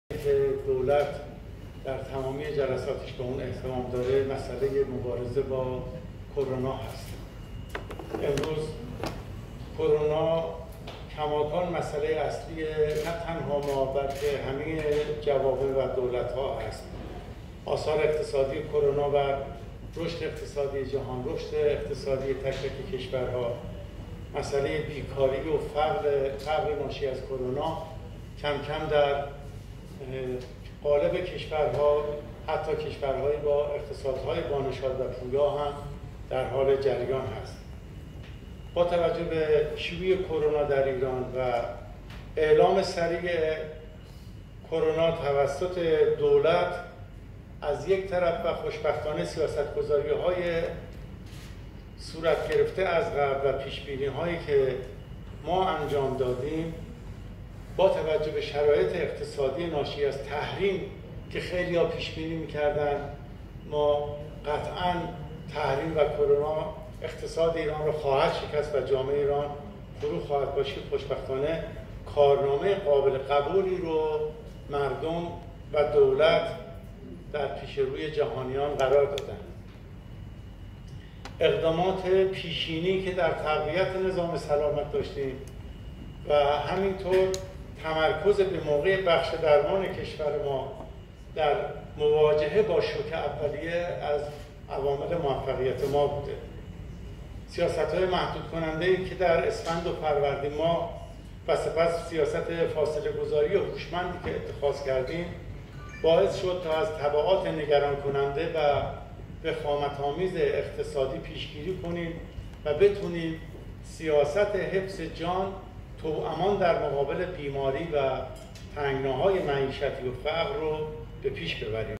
ربیعی: کرونا کماکان مسئله اول دولت است
ربیعی، سخنگوی دولت در نشست خبری:
کرونا کماکان مسئله اول دولت است
امروز کرونا کماکان مسئله اصلی جهان است و اقتصادهای برتر و با نشاط جهان نیز دچار مشکل شده‌اند
با توجه به شرایط اقتصادی فشار تحریم  و کرونا بسیاری تصور می‌کردند اقتصاد ایران فرو می‌ریزد؛ اما خوشبختانه کارنامه قابل قبولی را مردم و دولت در پیش روی جهانیان قرار دادند